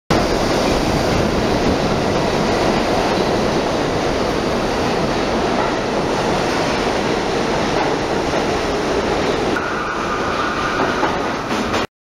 Canal Station Train Action NYC
New York J Trains Arriving and Leaving